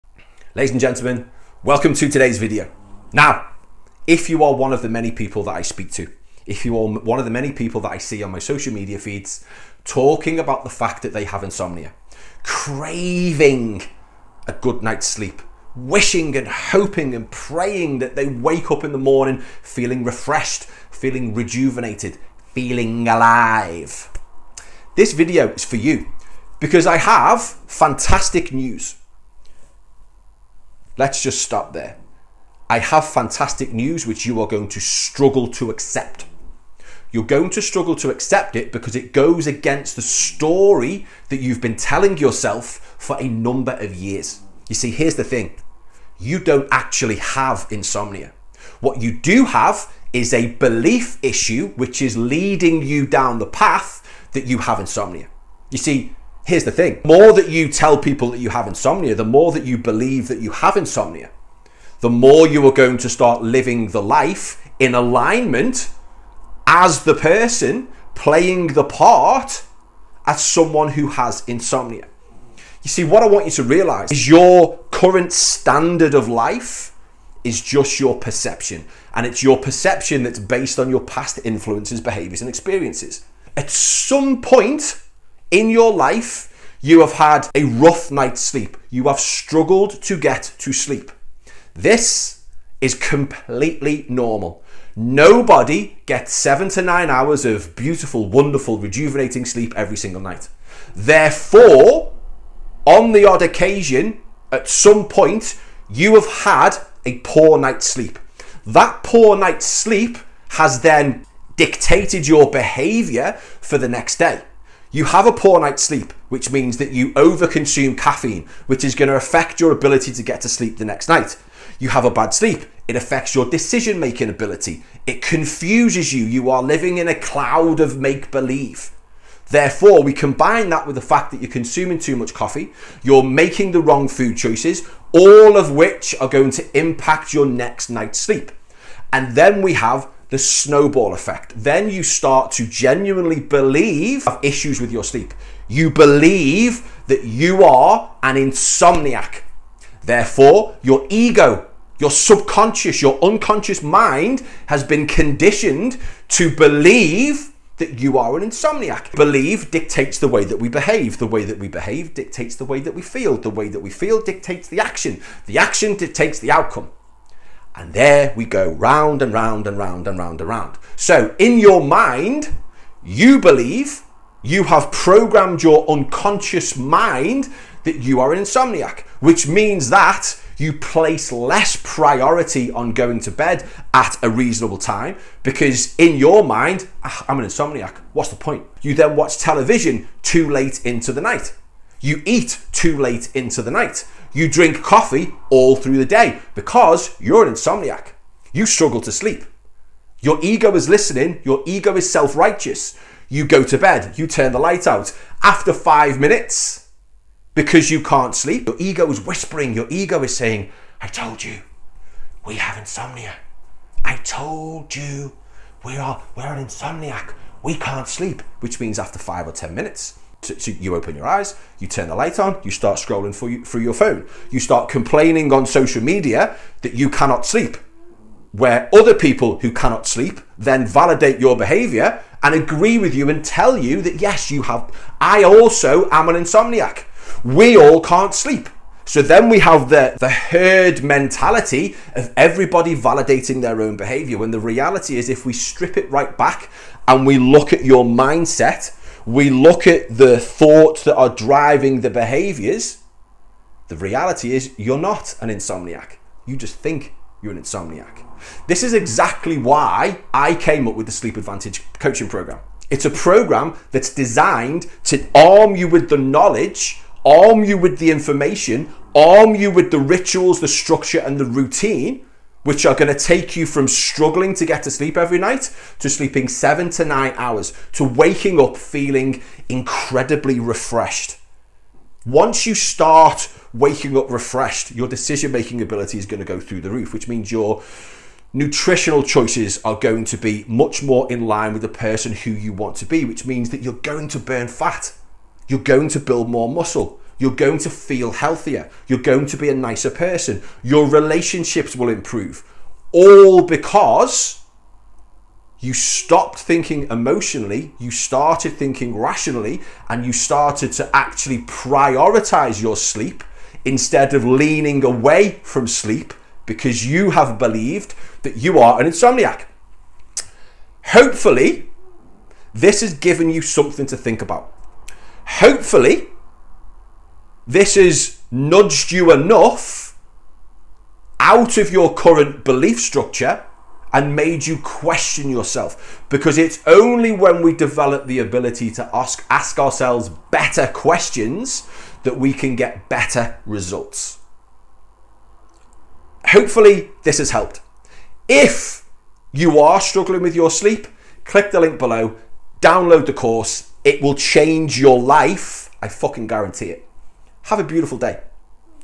Why Can't I Sleep (It's NOT Insomnia)
I have fantastic news, I know exactly why you can't sleep, and its nothing to do with you being an insomniac, you don't have insomnia, you just believe that you do, its not insomnia at all

Because you believe you have insomnia you now act in line with that belief so have the lifestyle and habits of someone who struggles to sleep

If this is something that's having a negative effect on your quality of life and you want the secret sauce to a supercharged life, click the link for instant access to my sleep advantage course -